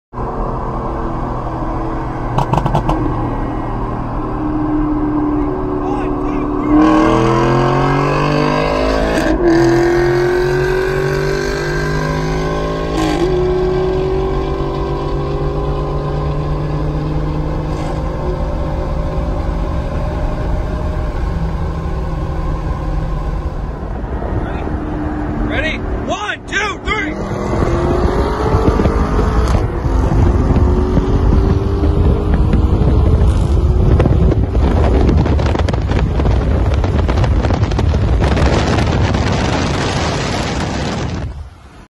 6th gen ZL1 Vs BMW M4
Manual ZL1 has an exhaust I think, maybe a tune idk. BMW has Bootmod stage 1 baby tune 93 octane and exhaust

Second clip submitted by: David M

*filmed in Pakistan*